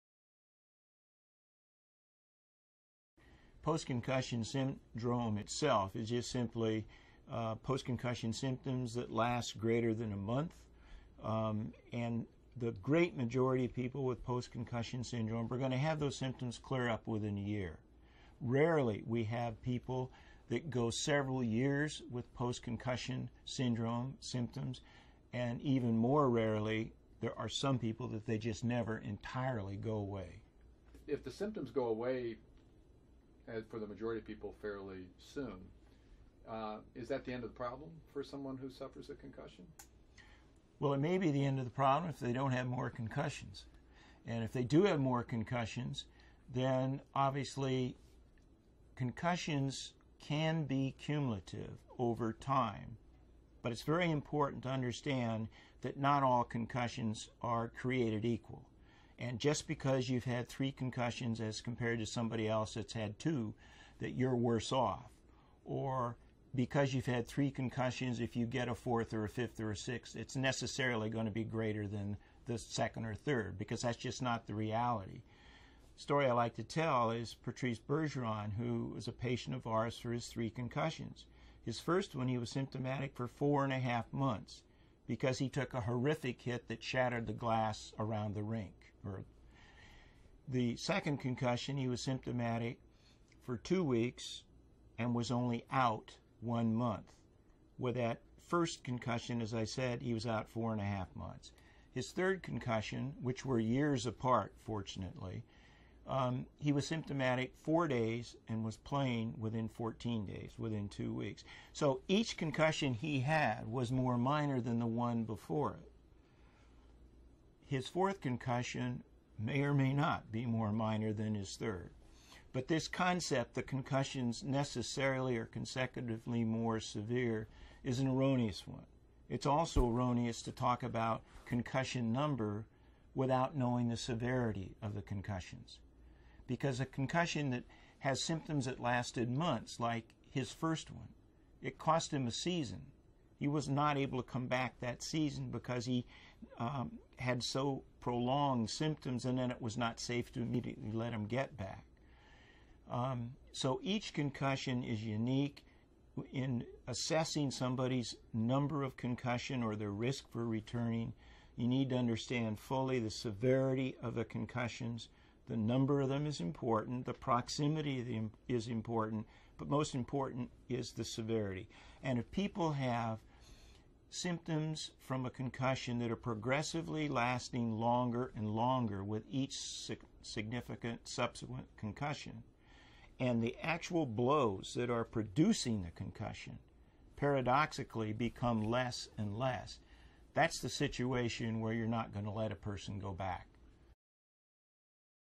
What is Post Concussion Syndrome?
Dr. Robert Cantu, Clinical Professor of Neurosurgery and Co-Director of the Center for the Study of Traumatic Encephalopathy at Boston University School of Medicine, discusses Post Concussion Syndrome, or concussion symptoms that persist for more than one month.